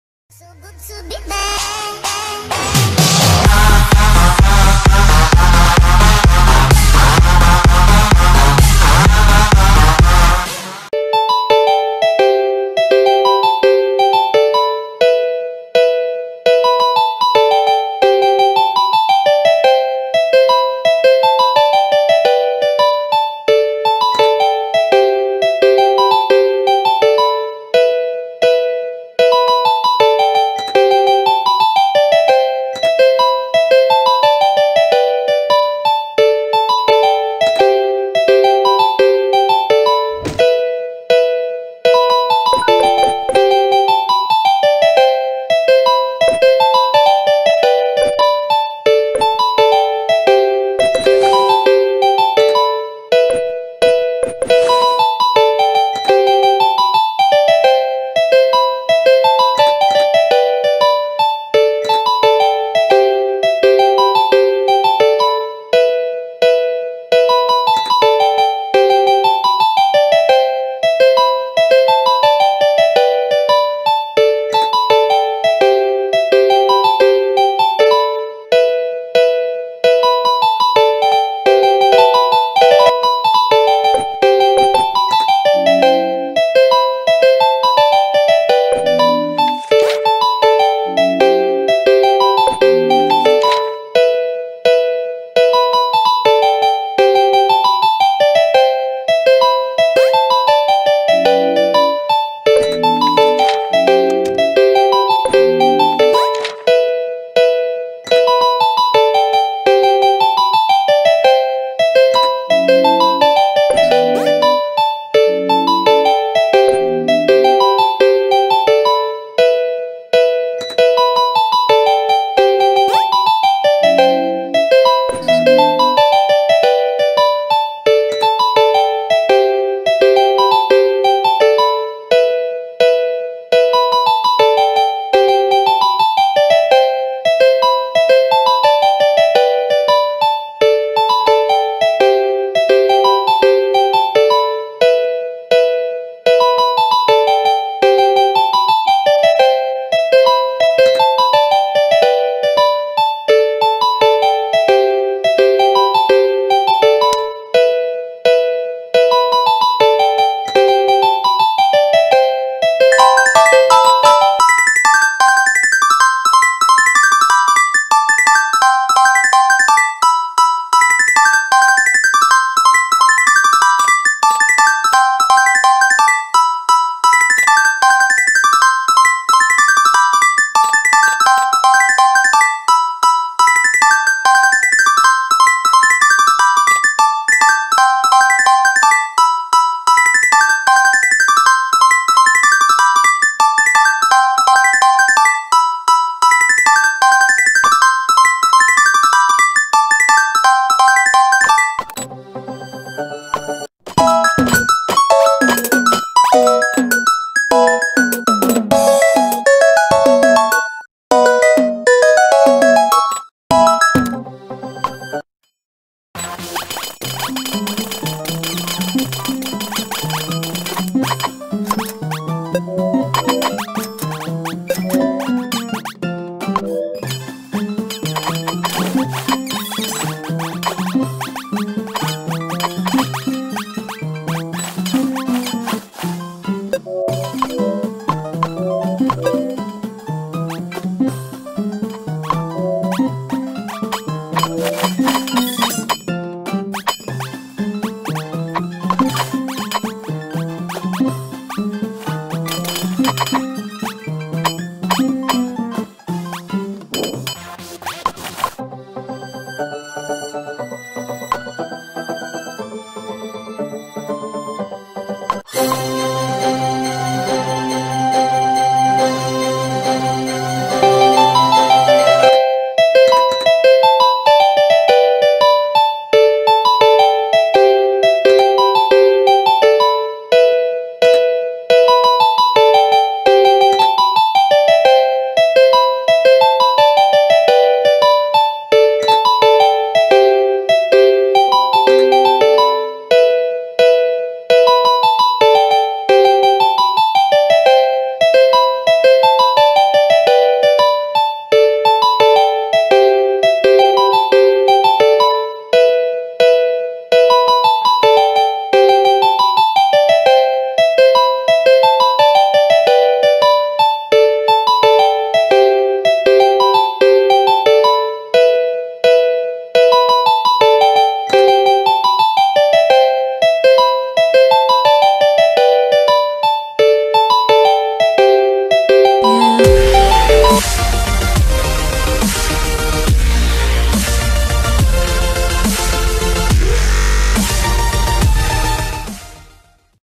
Back?
Hey guys! So I don't know if I should make youtube videos or not. I still play GT, but quit for awhile. No, I did not get scammed. Well another vid might be up soon! Thanks for staying here.